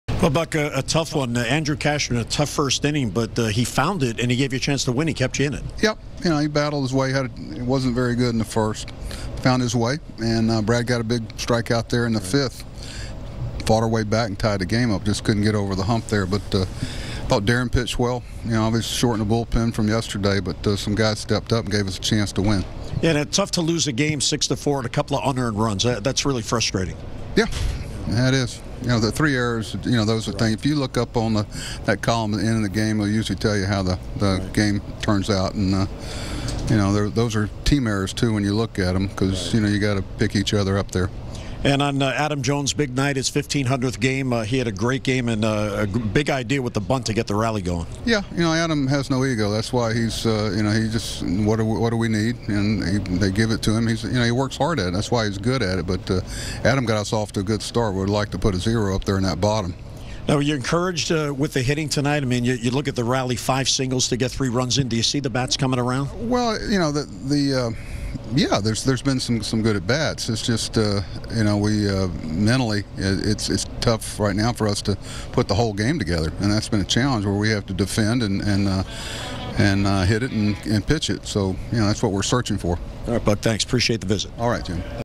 Buck Showalter on O's 6-4 loss to A's
The Orioles have lost 10 straight road games. More